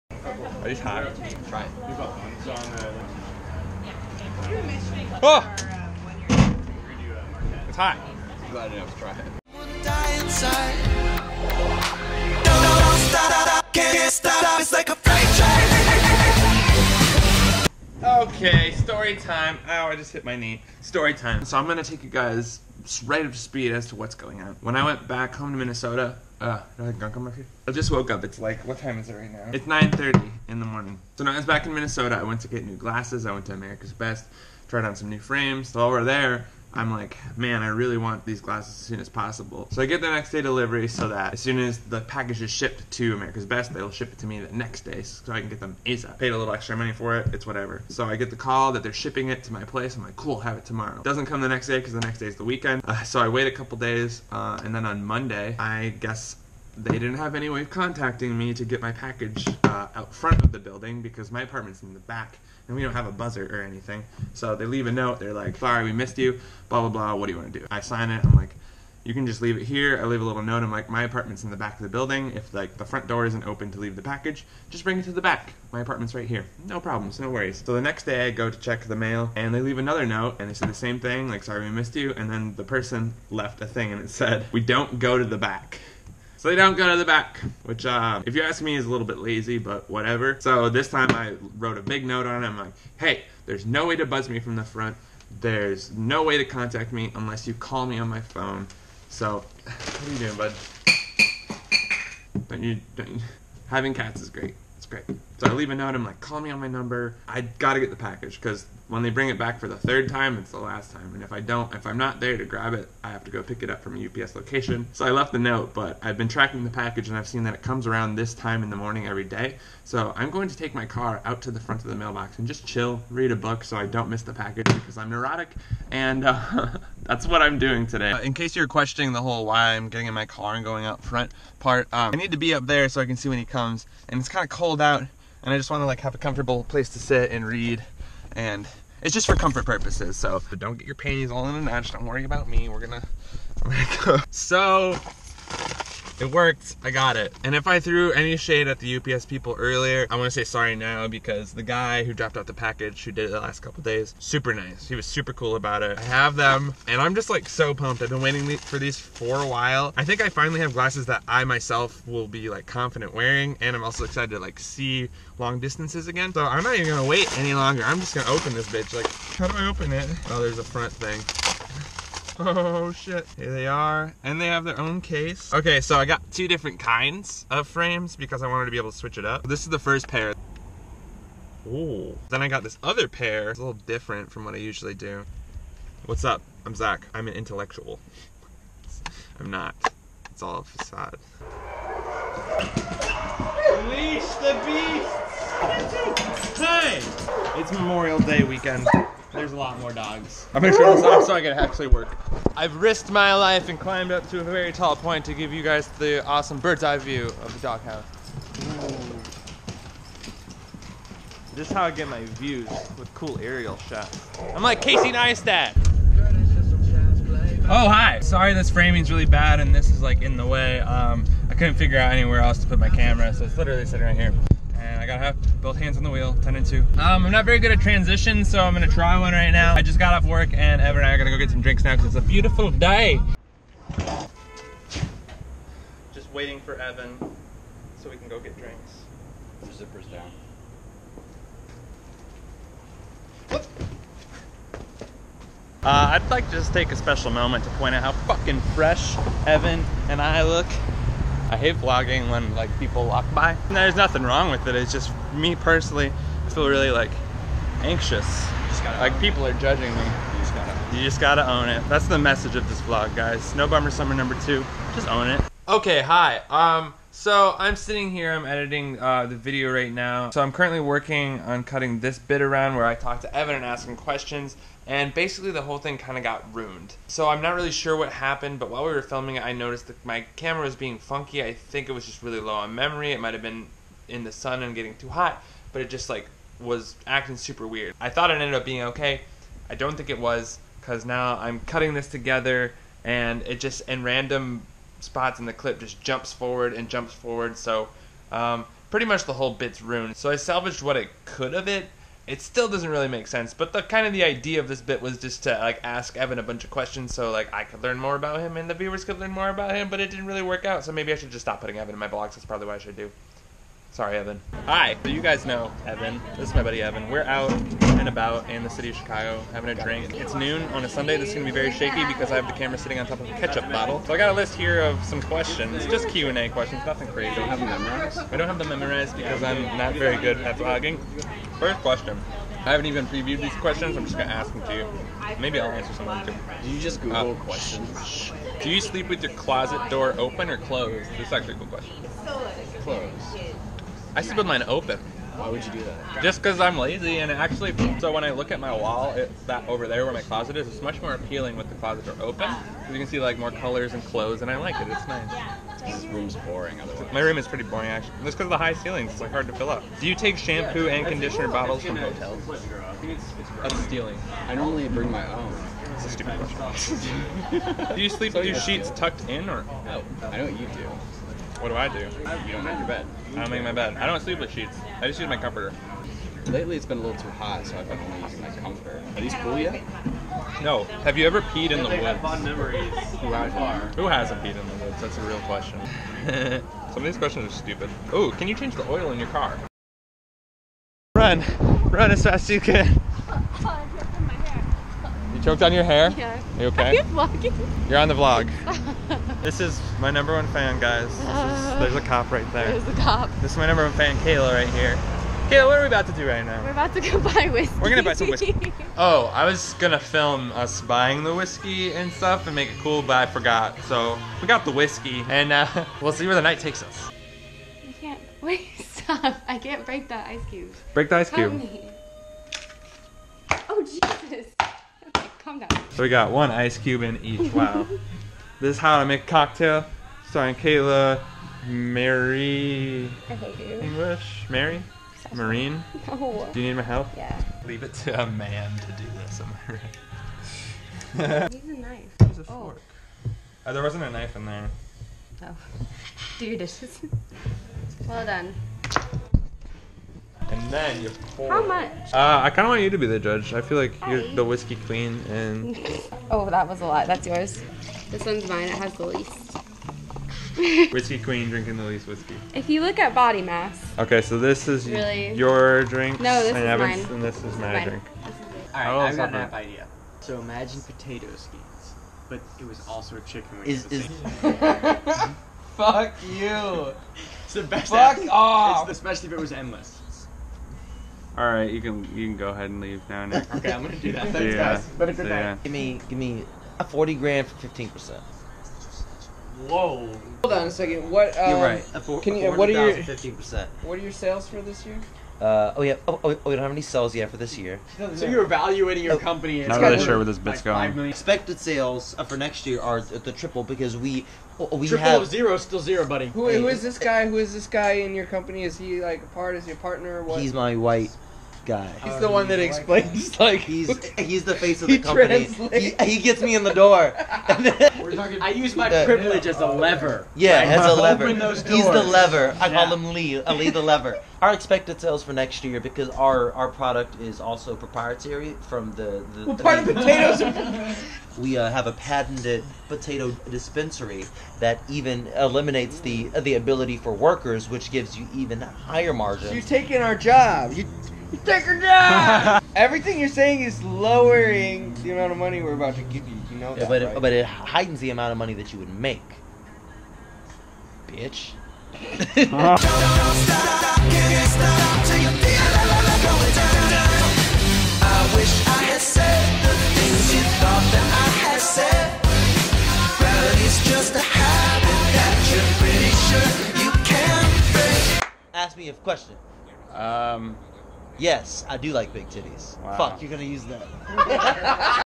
WHISKY CHAMPS - No Bummer Summer S1E2
Episode 2 of No Bummer Summer

Hunting down a UPS driver, interviewing my roommate, and making some Jack and Cokes

Clips shot between 5/24/17 - 5/28/17